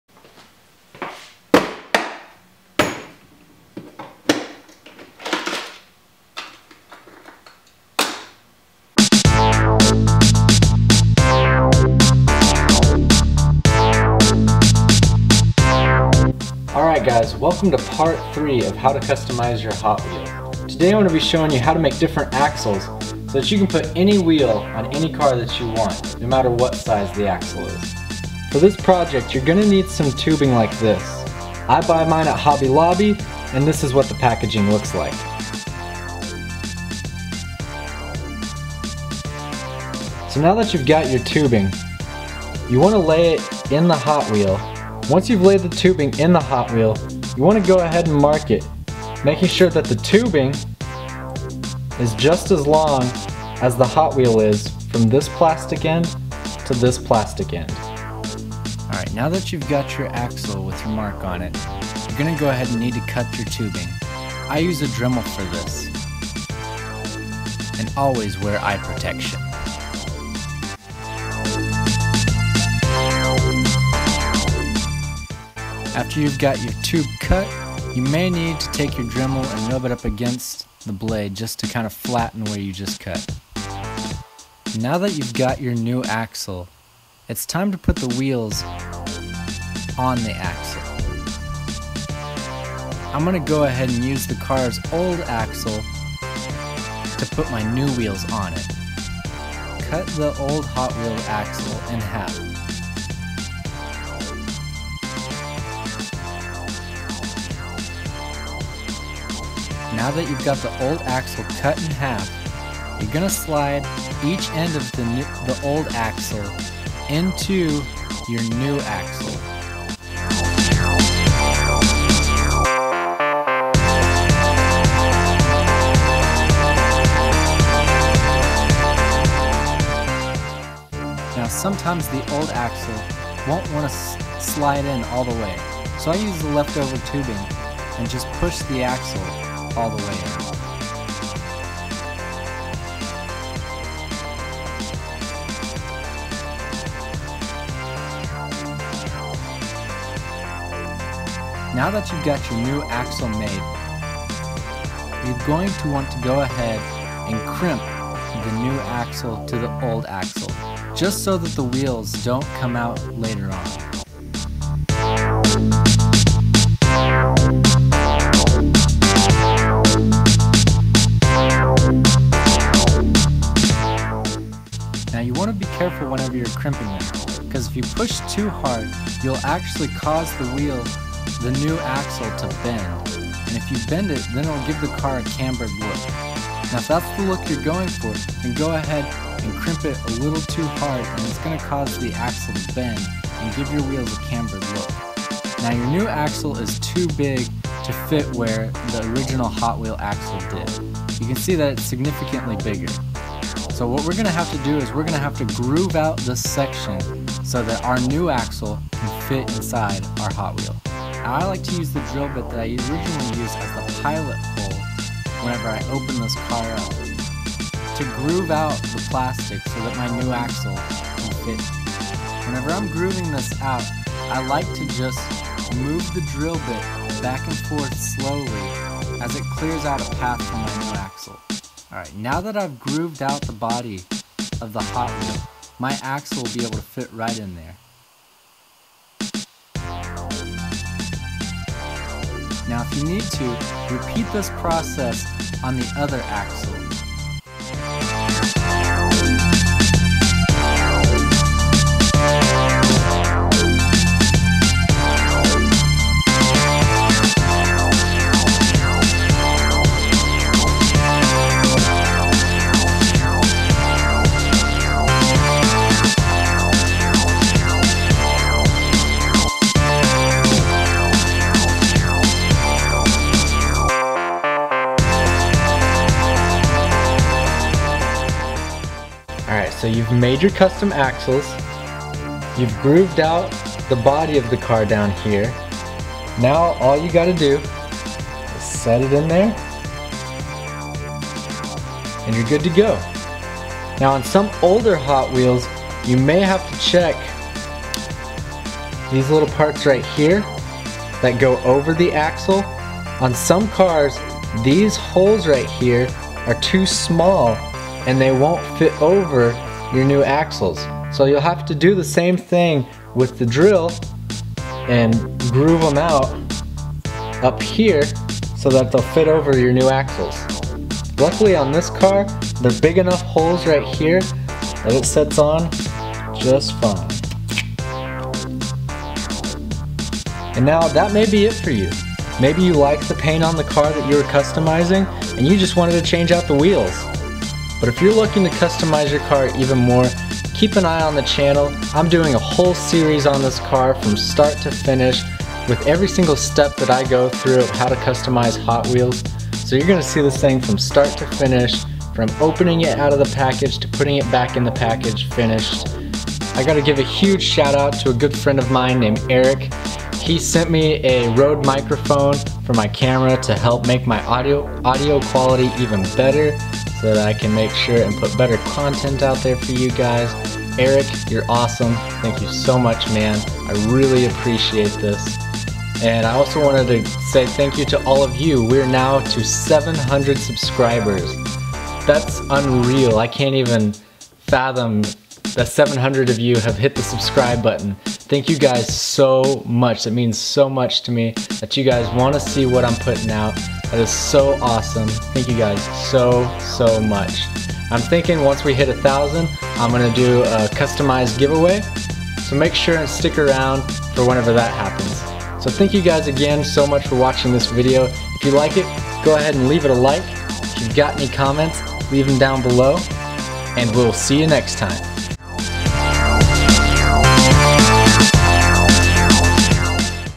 How to Customize HotWheels Part 3: How to Make Custom Axles
In this video I will show you how to make custom axles for your Hotwheels so that you can put any wheel on your custom Hotwheel! Enjoy!